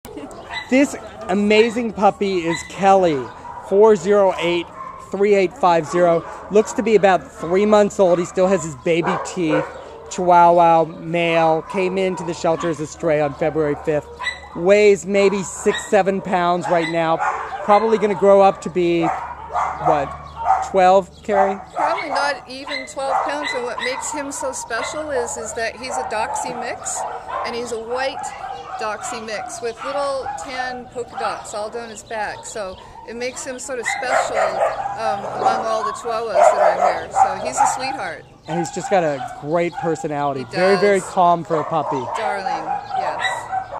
Kelly A4083850 ADOPTED!!!
Kelly is an amazing three month old white male Chihuahua/Dachshund mix puppy who came into the Baldwin Park shelter as a stray on February 5th.  Presently weighing five pounds this calm sweet puppy will probably grow to be approximately twelve pounds and loves to be held and cuddled.  Wonderful in every way Kelly will make a sensational addition to any family's household.